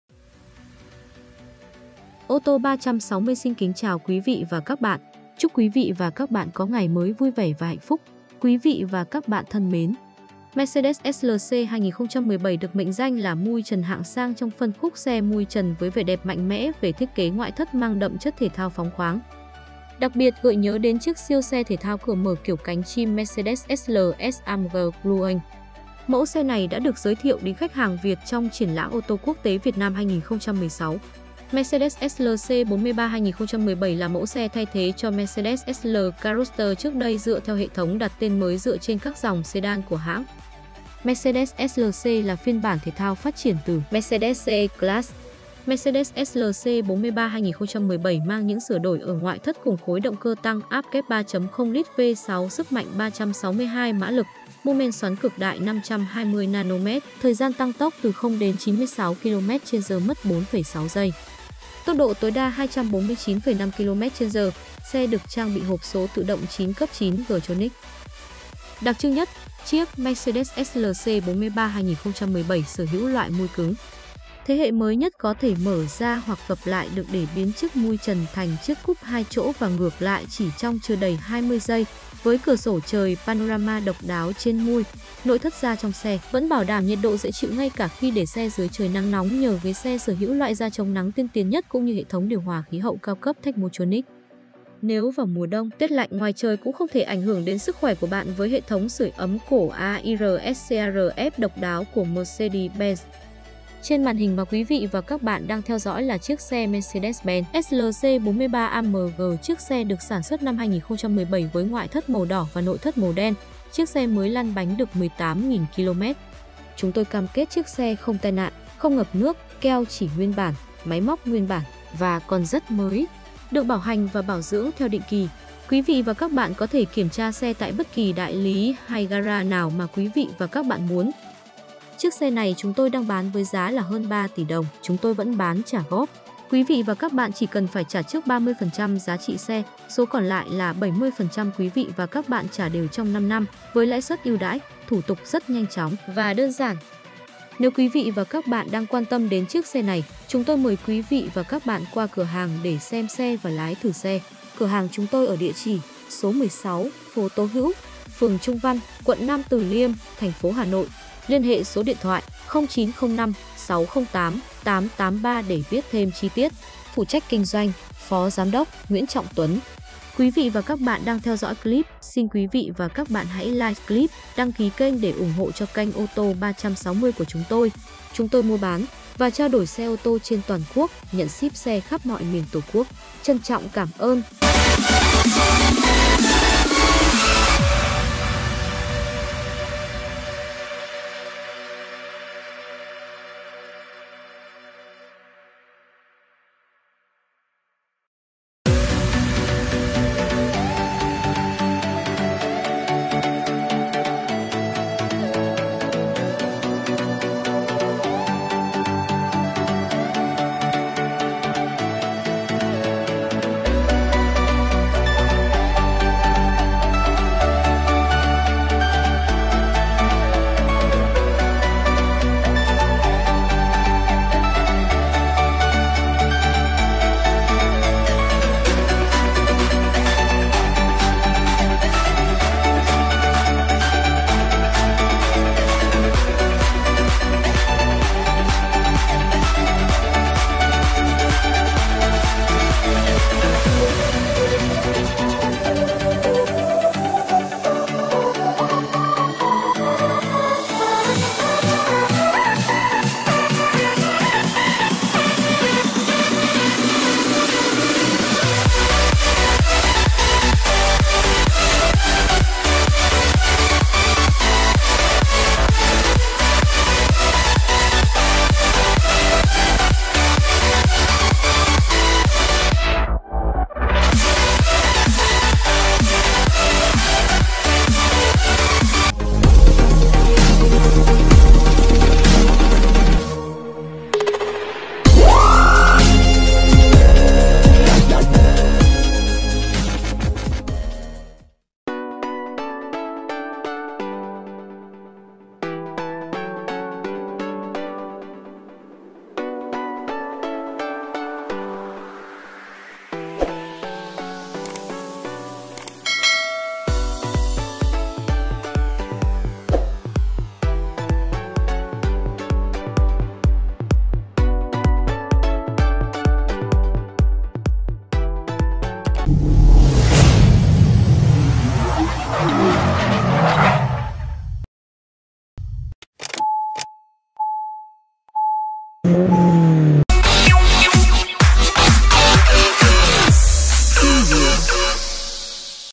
Như 1 con Robot biến hình xe thể thao mui trần rất đẹp Mercedes Benz SLC 43 AMG sản xuất năm 2017
Mercedes SLC 2017 được mệnh danh là mui trần hạng sang trong phân khúc xe mui trần với vẻ đẹp mạnh mẽ về thiết kế ngoại thất mang đậm chất thể thao phóng khoáng đặc biệt gợi nhớ đến chiếc siêu xe thể thao cửa mở kiểu cánh chim Mercedes SLS AMG Gullwing. Mẫu xe này sẽ được giới thiệu đến khách hàng Việt trong triển lãm Ô Tô Quốc Tế Việt Nam 2016 (VIMS 2016) được diễn ra từ 26 đến 30 tháng 10.
Mercedes SLC 43 2017 là mẫu xe thay thế cho Mercedes SLK Roadster trước đây dựa theo hệ thống đặt tên mới dựa trên các dòng sedan của hãng. Mercedes SLC là phiên bản thể thao phát triển từ Mercedes C-class. Mercedes SLC 2017 mang những sửa đổi ở ngoại thất cùng khối động cơ tăng áp kép 3.0 lít V6 sức mạnh 362 mã lực, mô-men xoắn cực đại 520 Nm. Thời gian tăng tốc 0-96 km/h mất 4,6 giây với Mercedes SLC43, tốc độ tối đa 249,5 km/h, xe được trang bị hộp số tự động 9 cấp 9G-Tronic.
Đặc trưng nhất, chiếc Mercedes SLC 43 sở hữu loại mui cứng thế hệ mới nhất có thể mở ra hoặc gập lại được để biến chiếc mui trần thành chiếc coupe 2 chỗ và ngược lại chỉ trong chưa đầy 20 giây, với cửa sổ trời panorama độc đáo trên mui, nội thất da trong xe vẫn bảo đảm nhiệt độ dễ chịu ngay cả khi để xe dưới trời nắng nóng nhờ ghế xe sở hữu loại da chống nắng tiên tiến nhất cũng như hệ thống điều hòa khí hậu cao cấp Thermotronic. Nếu vào mùa đông, tiết lạnh ngoài trời cũng không thể ảnh hưởng đến sức khỏe của bạn với hệ thống sưởi ấm cổ AIRSCARF độc đáo của Mercedes-Benz.